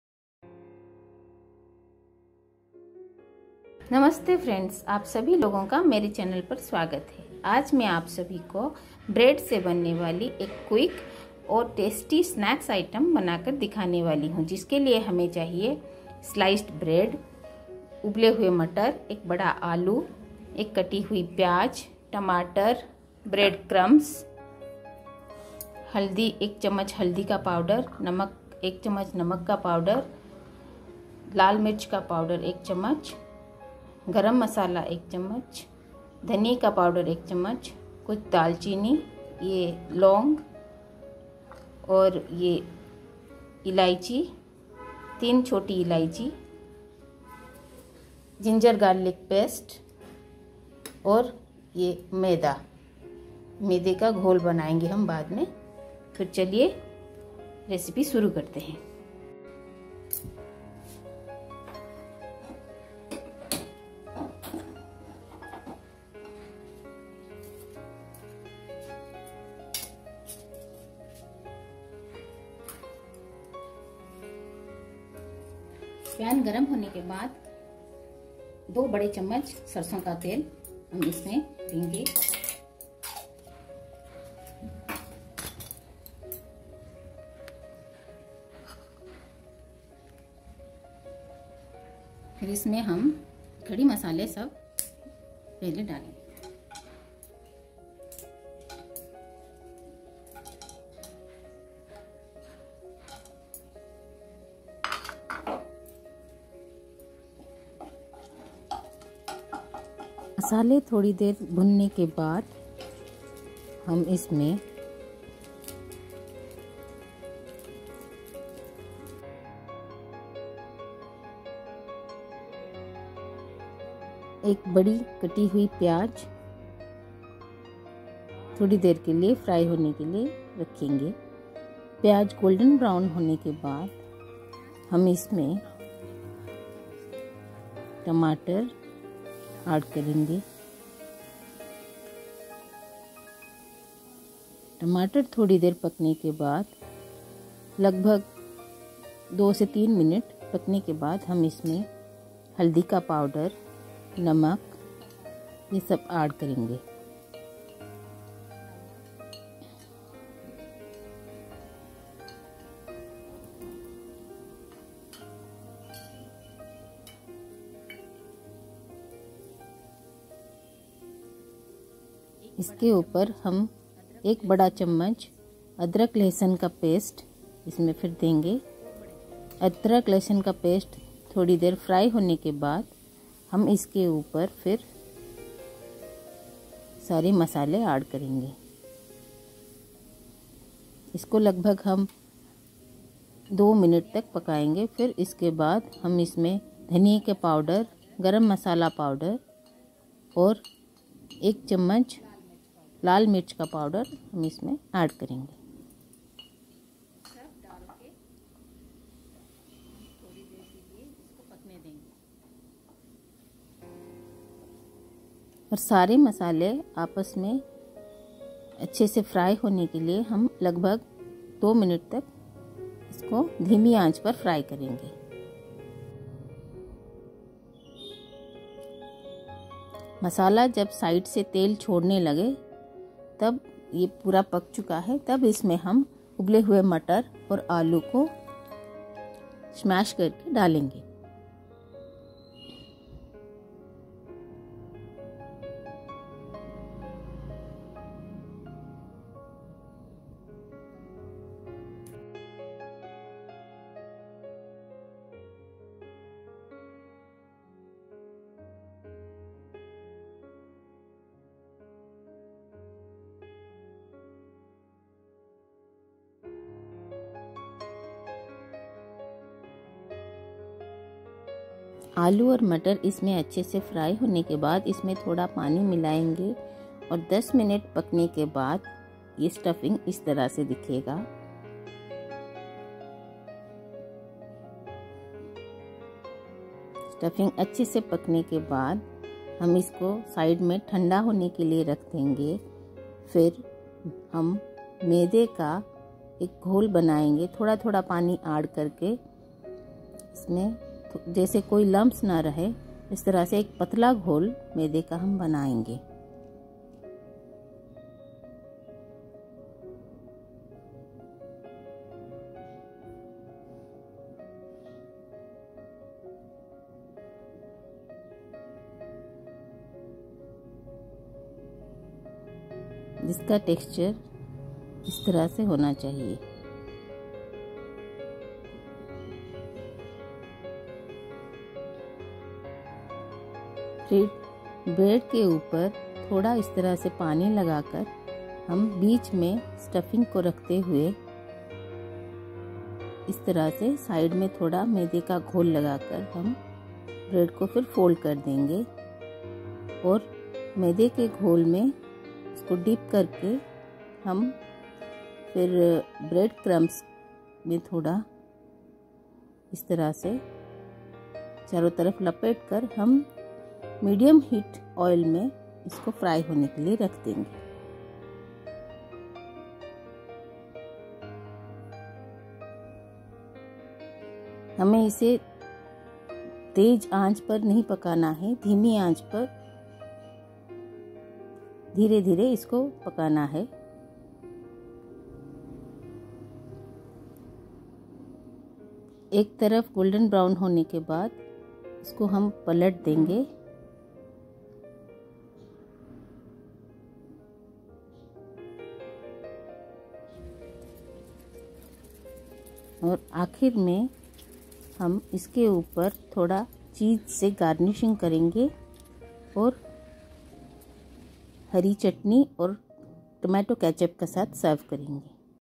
Easy bread pocket making.Evening snack item made from bread.
In today's video I tried to make an easy evening snack item from bread. If you have any quarry regarding this recipe then you can ask me in comment section.